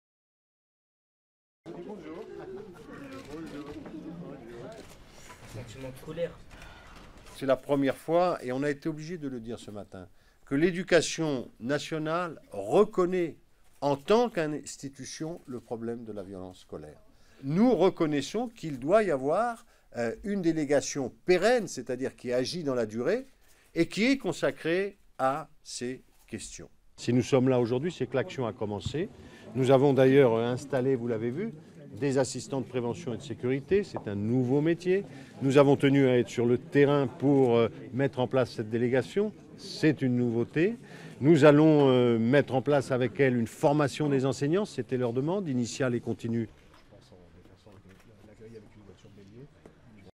Violences scolaires: Peillon lance une mission de lutte
Le ministre de l'Education nationale, Vincent Peillon, a annoncé lundi 12 novembre à Amiens l'installation officielle d'une délégation ministérielle sur la violence scolaire, institution "pérenne" pour "briser le silence" et trouver des solutions à court et à long terme. Durée: 00:50